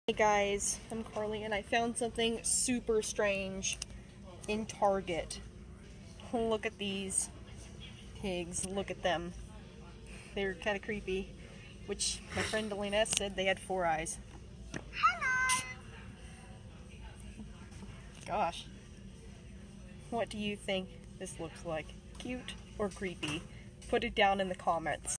Found something super strange in Target
Pipsqueak and I were at Target and we found something super strange

We don’t own anything in this video

Check out Jett’s tv here:

Check out kimmi the clown here:

All ages, happy watching